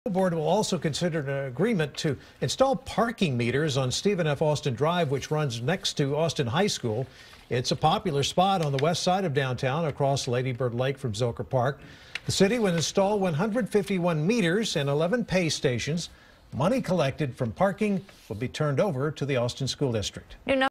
Austin ISD considers parking meters along Stephen F. Austin Drive
The city would install 151 meters and 11 pay stations.